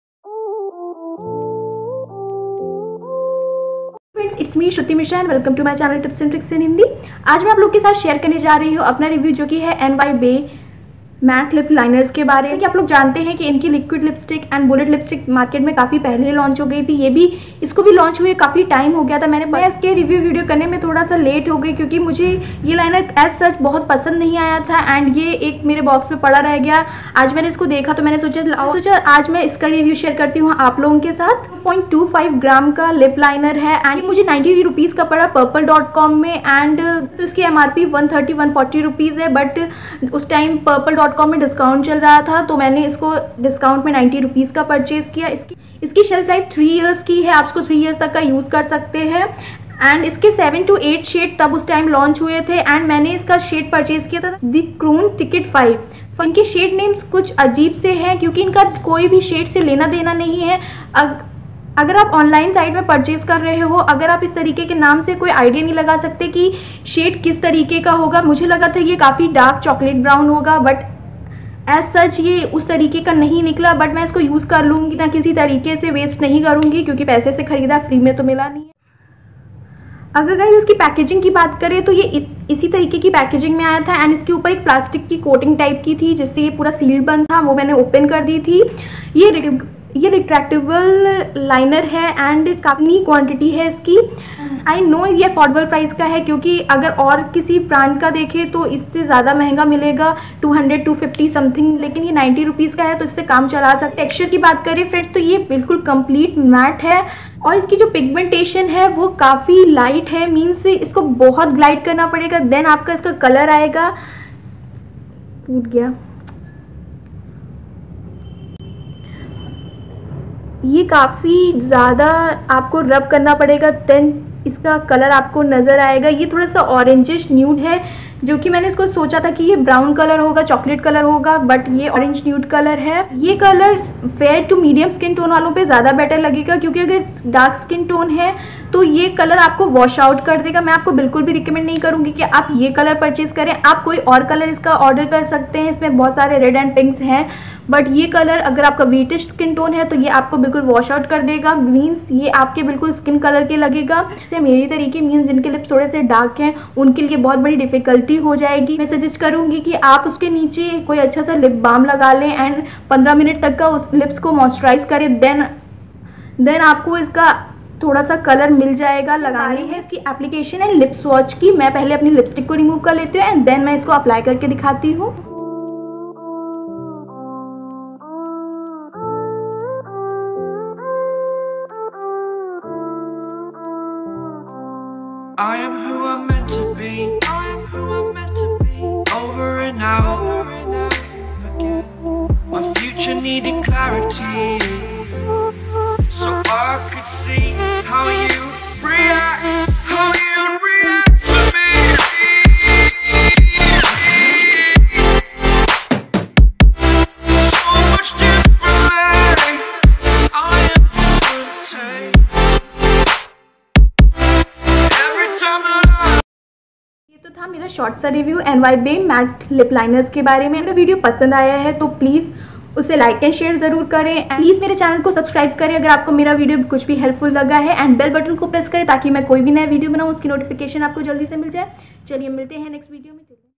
Ny Bae Lipliner Review & Swatch |Shade - 5| By Tips And Tricks In Hindi
Today my video is on Ny Bae Lip liner Review And Swatch...

Shruti Mishra
Xoxo
Tips And Tricks In Hindi
Tipsandtricksinhindi